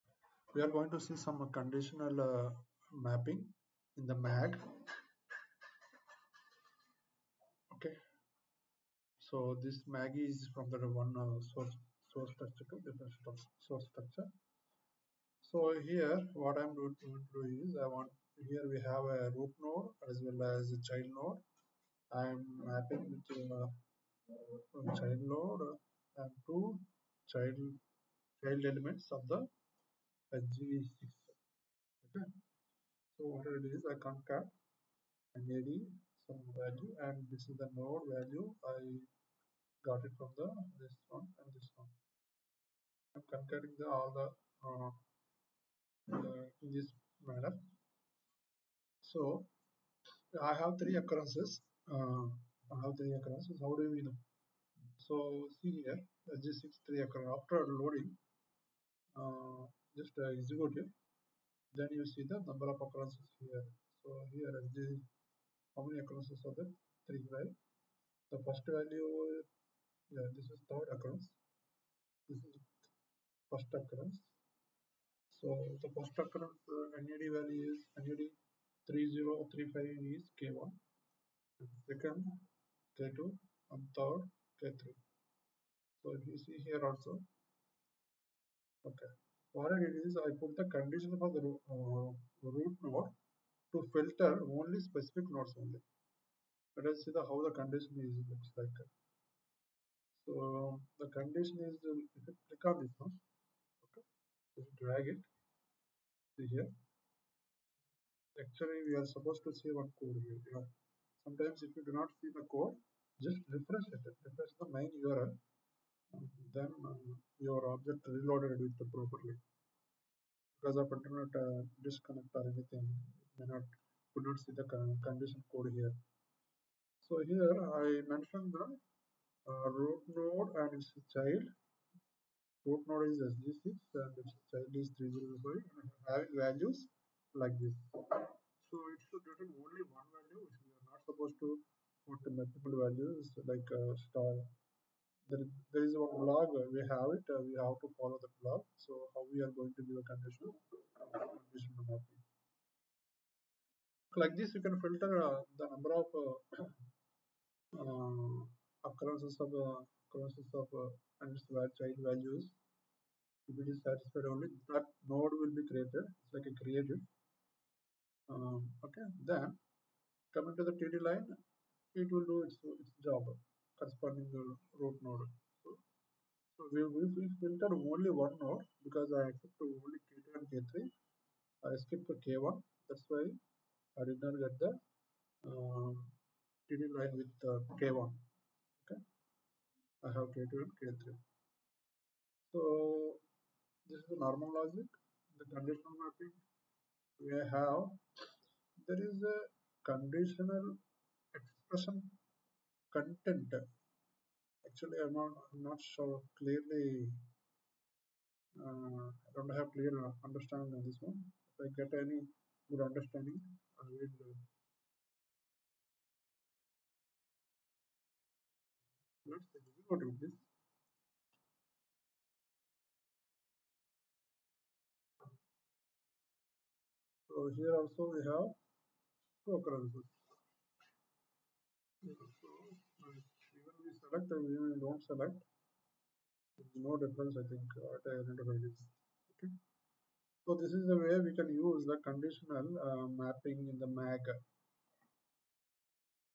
SAP TPM MAG conditional mapping target node it's child
Comment syntax
(: This is an comment inside XPath expression :)
When you map a source group node to a target group node. And then map a source leaf node to the same target group node. It automatically creates a “conditional mapping”. What is this feature? Let’s have a look at it.

drop email if any training on SAP TPM integration advisor; dpothireddi at gmail